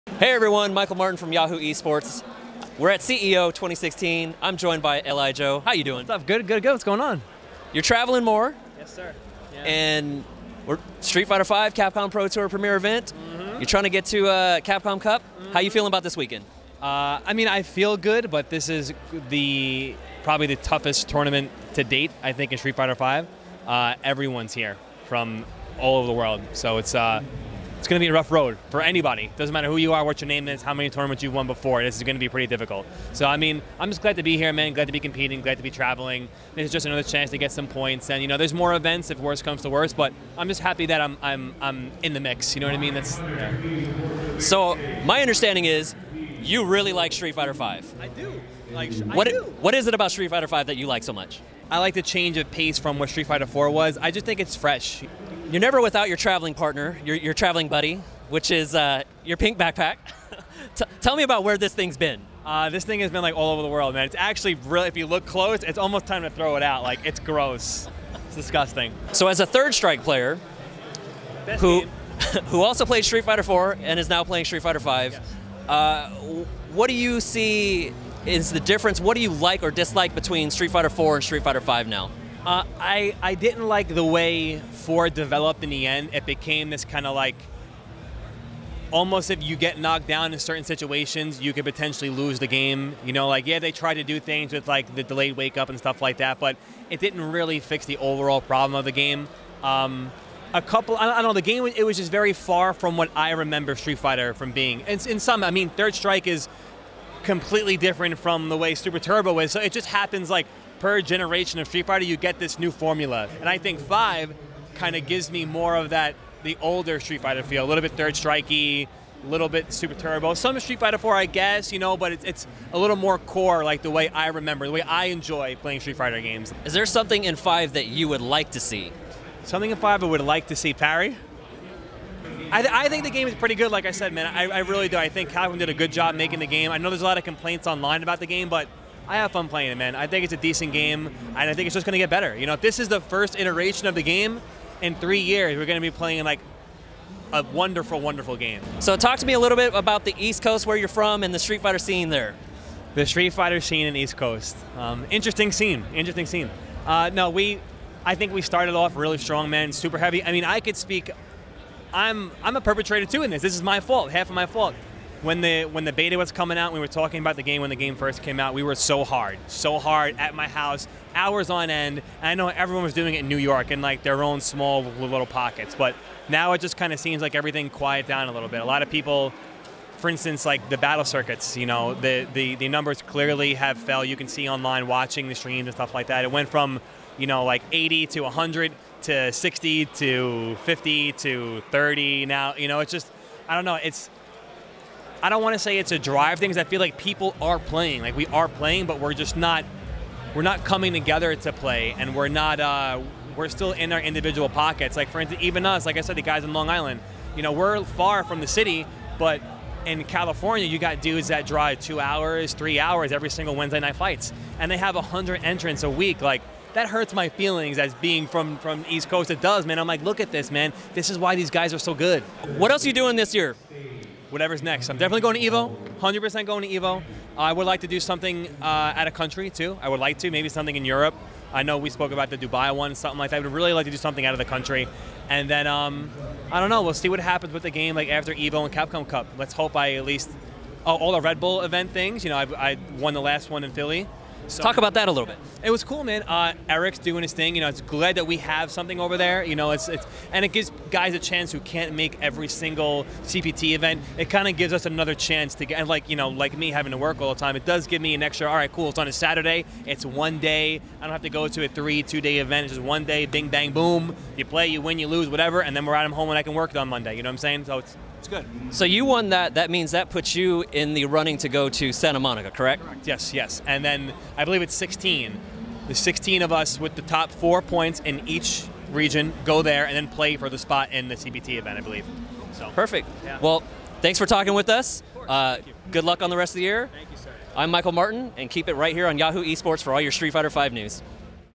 LI Joe on the Capcom Pro Tour and Street Fighter Vs fresh change of pace
Joe LI Joe Ciaramelli speaks with Michael Martin about what makes Street Fighter V a breath of fresh air. For more in-depth coverage of competitive gaming, visit Yahoo Esports.